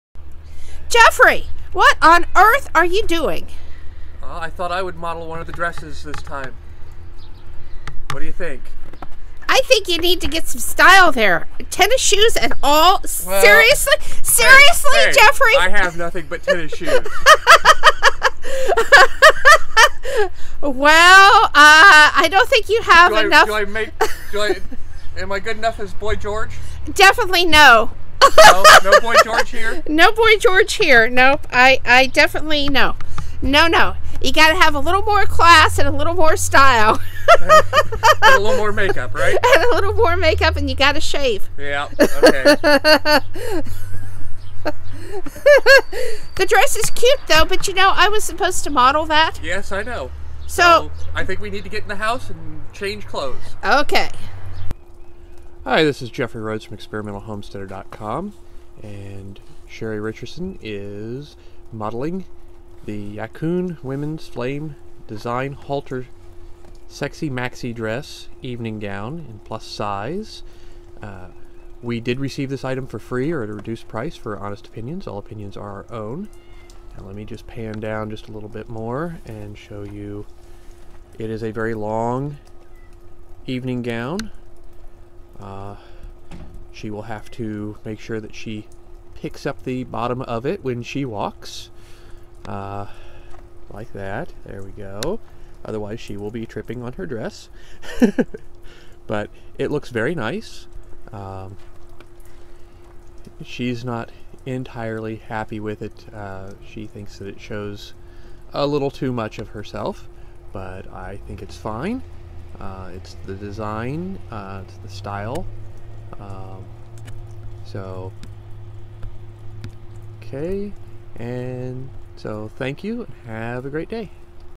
Yacun Flame Design Evening Gown Review
Sheri Ann Richerson from ExperimentalHomesteader.com models the Yacun Flame Design Evening Gown - well - after Jeffrey Rhoades models it first!

This is a very nice dress - ideal for evening wear - but be warned it is very low cut in the front. I tried tightening the straps but it pulled the top too high. There is padding in the top which was nice. I liked the elastic gathered waist. It was a bit long for me however. I am 5 foot 1 inch. I think it would be easy to hem or I could just wear a taller pair of heels.

The colors are very vibrant however it does not come with care instructions. We have yet to try to wash it and I imagine I will wash it in the delicate cycle and hang it to dry.

The fabric is really soft. The dress flows nicely and is easy to put on and take off. The seams are all intact.

For the right event, this dress is perfect!

Click here to purchase the Yacun Flame Design Evening Gown - please note this is an affiliate link and I do get a small percentage if you purchase this item using this link.